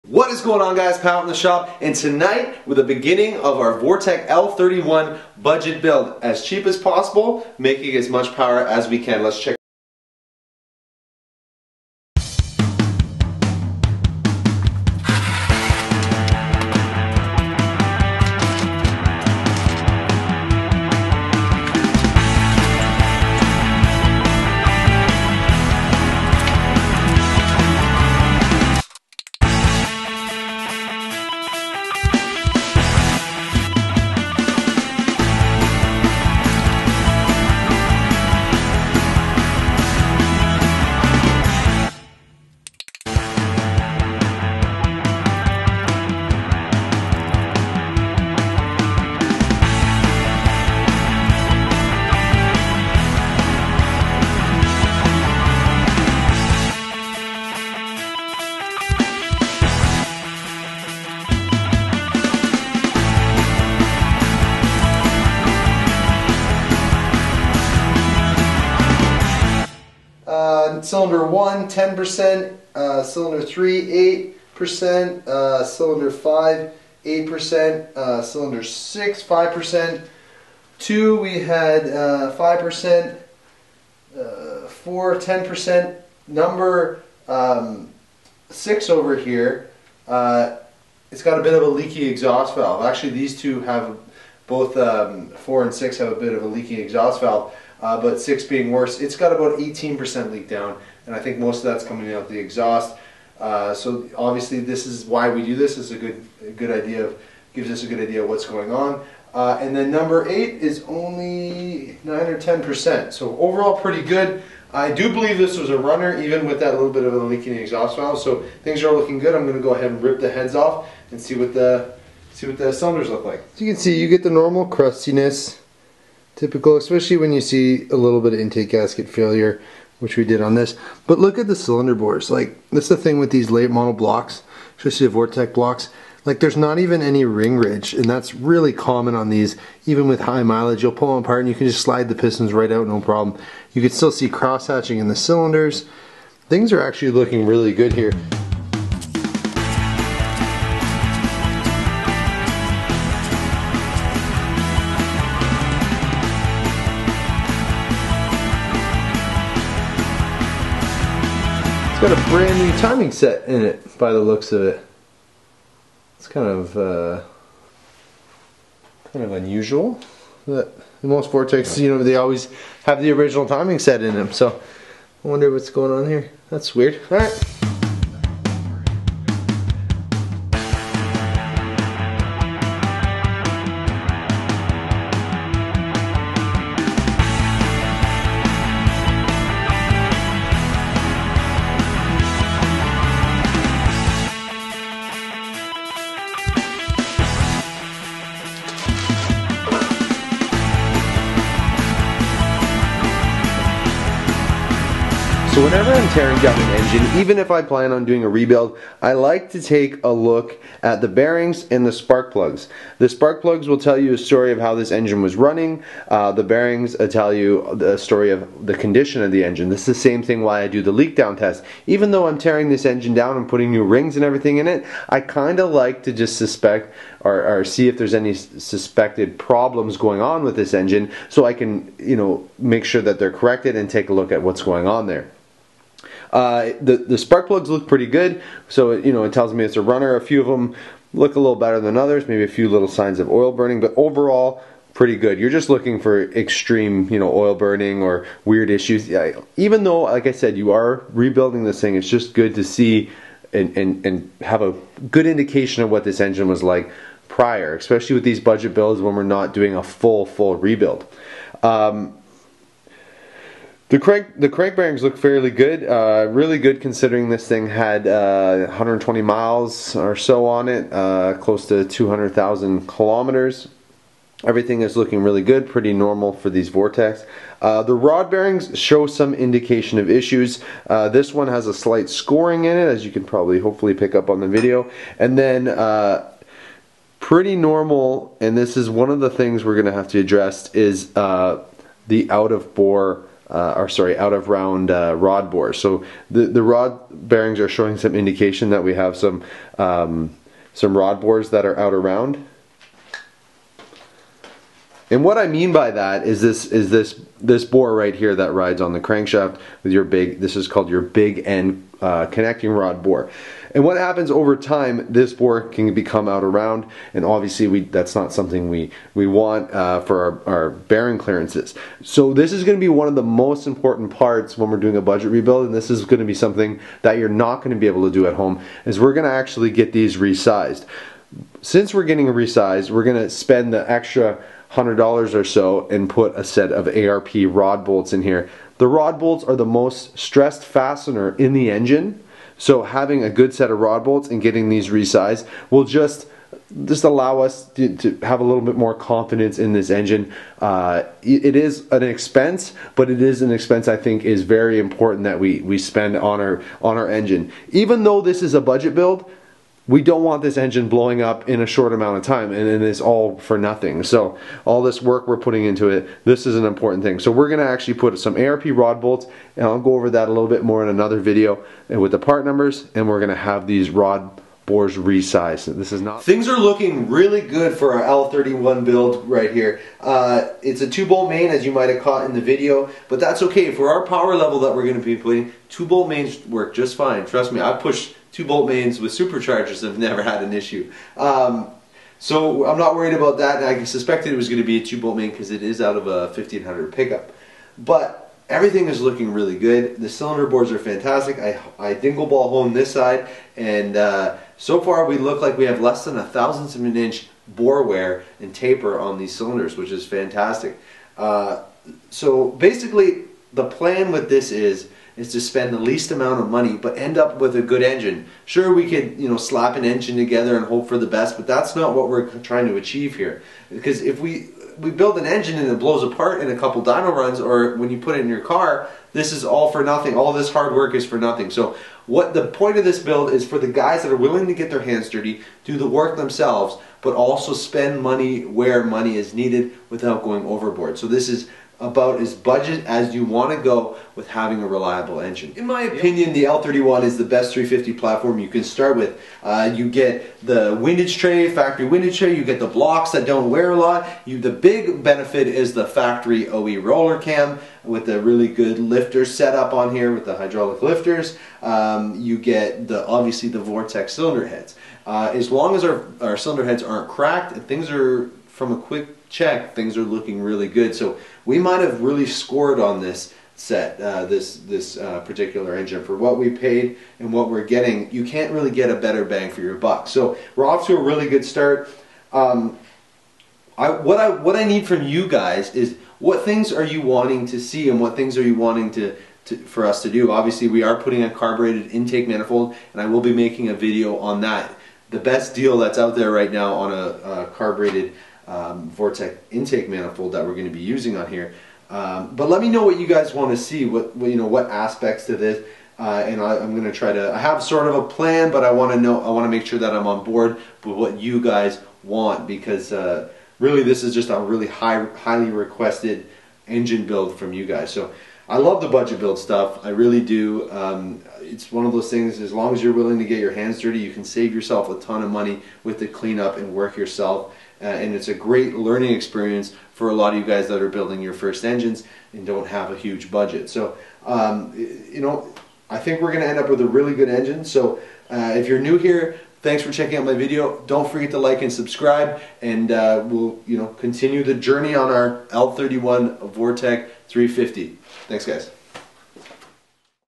Low Buck 350 Vortec Build! ( L31 Tear down) Part 1
Everyone loves a budget build! Shooting for the best HP per dollar we can get out of $300 L31 5.7 Vortec engine, lets tear it down, clean it up and do a budget refresh. Performance upgrades and head porting will follow! The final engine will be dynoed